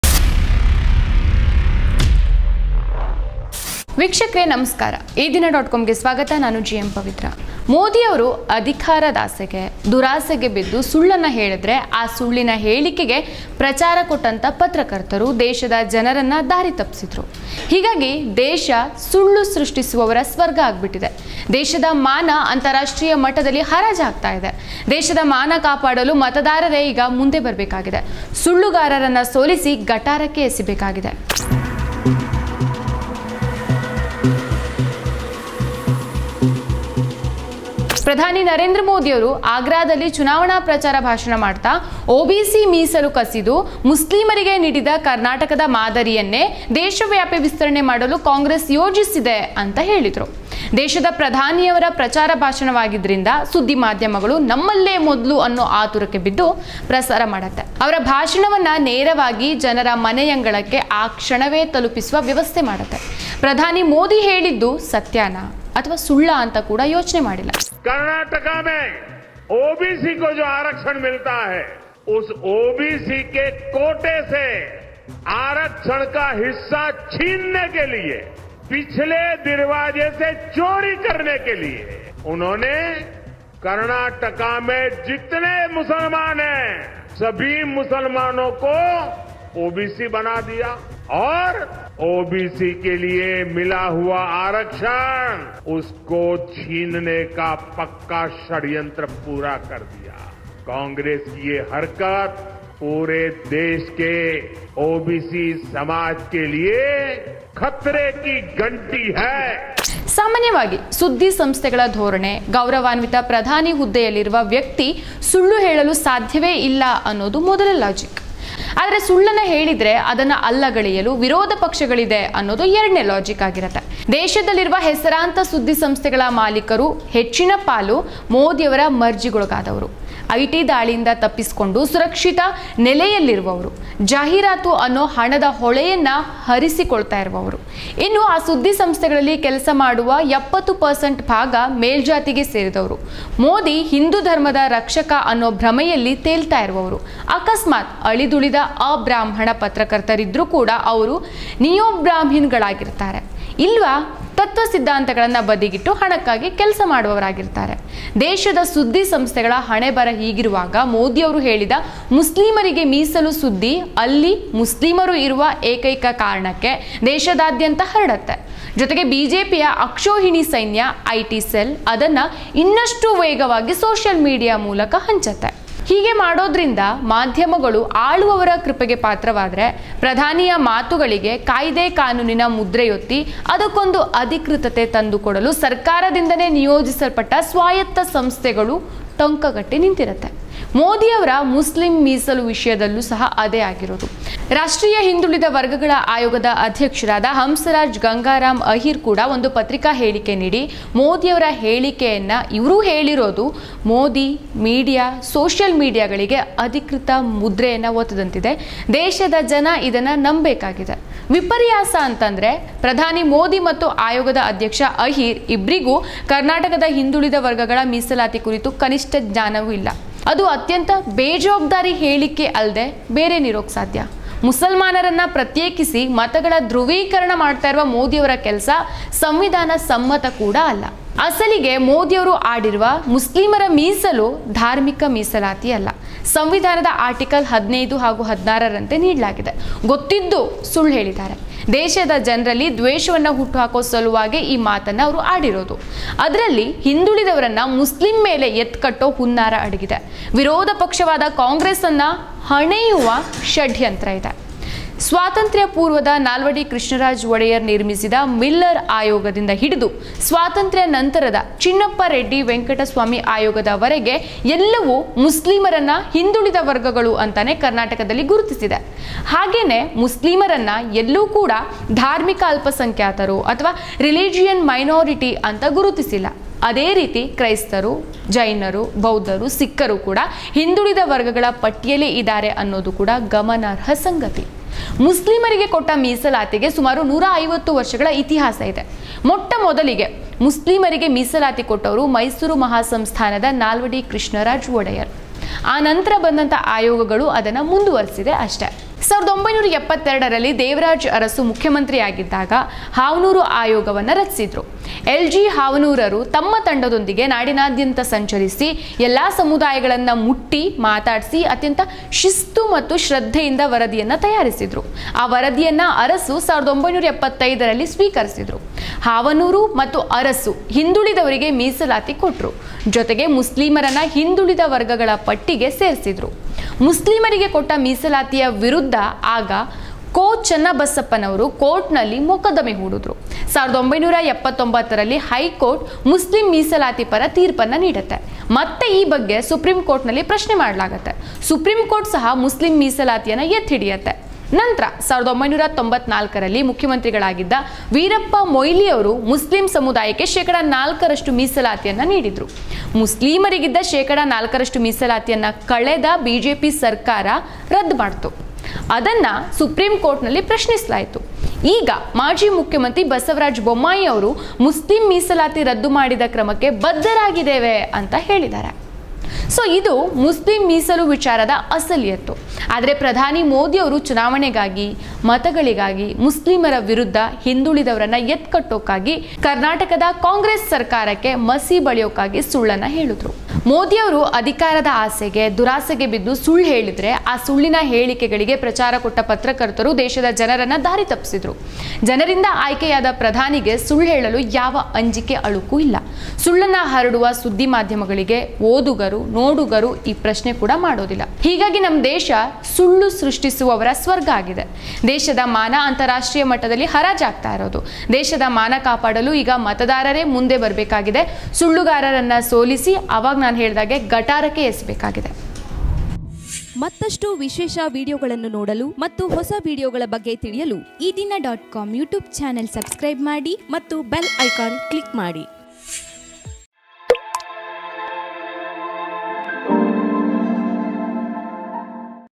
ಮೋದಿಯವರು ಹೇಳಿದ ‘ಮುಸ್ಲಿಂ ಮೀಸಲು’ ವಿಷಯ ಅಪ್ಪಟ ಸುಳ್ಳು
ಮೋದಿಯವರು ಅಧಿಕಾರದಾಸೆಗೆ, ದುರಾಸೆಗೆ ಬಿದ್ದು ಸುಳ್ಳು ಹೇಳಿದರೆ, ಆ ಸುಳ್ಳಿನ ಹೇಳಿಕೆಗಳಿಗೆ ಪ್ರಚಾರ ಕೊಟ್ಟ ಪತ್ರಕರ್ತರು, ದೇಶದ ಜನರನ್ನು ದಾರಿ ತಪ್ಪಿಸಿದರು. ಹೀಗಾಗಿ ದೇಶ ಸುಳ್ಳು ಸೃಷ್ಟಿಸುವವರ ಸ್ವರ್ಗವಾಗಿದೆ. ದೇಶದ ಮಾನ ಅಂತಾರಾಷ್ಟ್ರೀಯ ಮಟ್ಟದಲ್ಲಿ ಹರಾಜು ಆಗುತ್ತಿದೆ. ದೇಶದ ಮಾನ ಕಾಪಾಡಲು ಮತದಾರರೇ ಮುಂದೆ ಬರಬೇಕಾಗಿದೆ, ಸುಳ್ಳುಗಾರರನ್ನು ಸೋಲಿಸಿ ಗಟಾರಕ್ಕೆ ಎಸೆಯಬೇಕಾಗಿದೆ. ಮೋದಿ ಹೇಳಿದ ಸುಳ್ಳುಗಳ ಸರಣಿಯ ಒಂದು ತುಣುಕು ಈ ವಿಡಿಯೋದಲ್ಲಿದೆ.